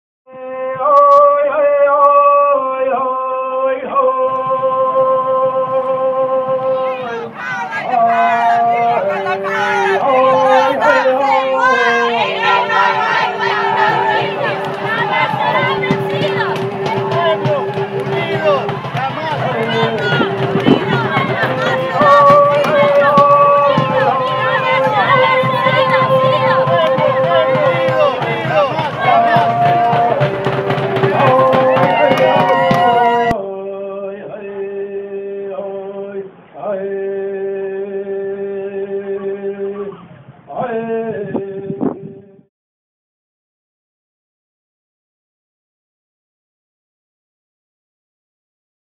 Greg Walden - It's Time for Immigration Reform
Residents of Oregon's Congressional District 2 march 44 miles from Madras, Oregon to Bend, Oregon to deliver thousands of hand-signed petitions asking Greg Walden, their representative, to listen to District 2's needs. They elected him; not John Boehner.